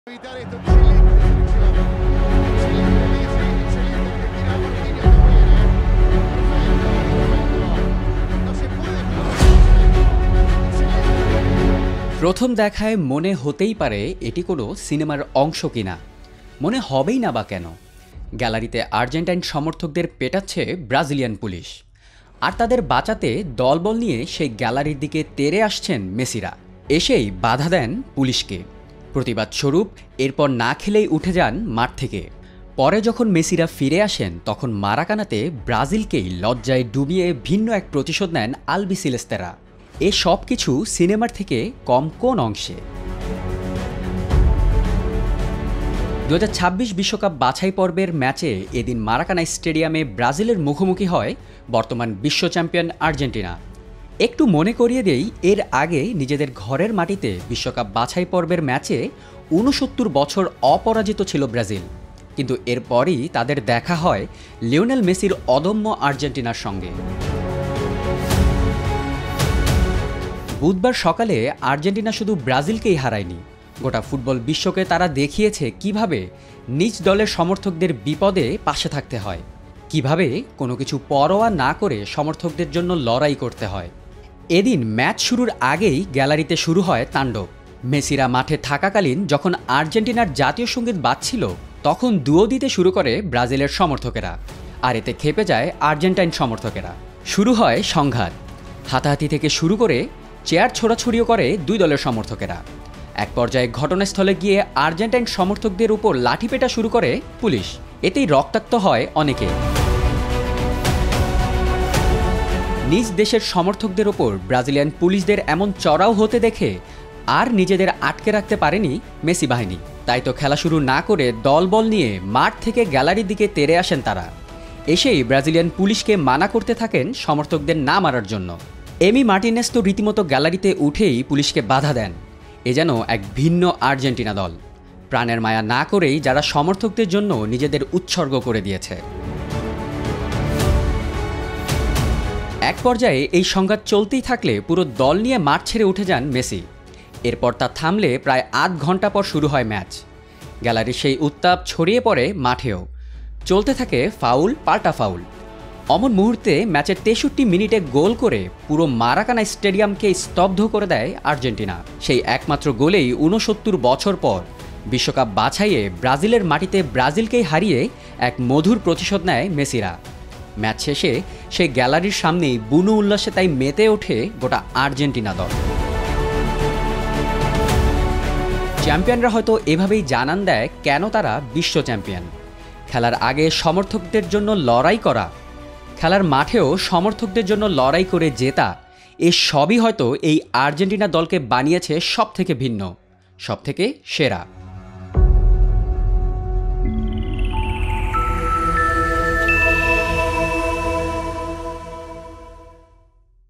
মারাকানার দাঙ্গায় সমর্থকদের জন্য জীবন বাজি রাখতেও পরোয়া করেনি আর্জেন্টিনা | TBS Sports
Argentina players clash with Brazilian policeman to protect fans in Maracana
বুধবার সকালে আর্জেন্টিনা শুধু ব্রাজিলকেই হারায় নি, গোটা ফুটবল বিশ্বকে তারা দেখিয়েছে কীভাবে নিজ দলের সমর্থকদের বিপদে পাশে থাকতে হয়। কীভাবে কোন কিছু পরোয়া না করে সমর্থকদের জন্য লড়াই করতে হয়।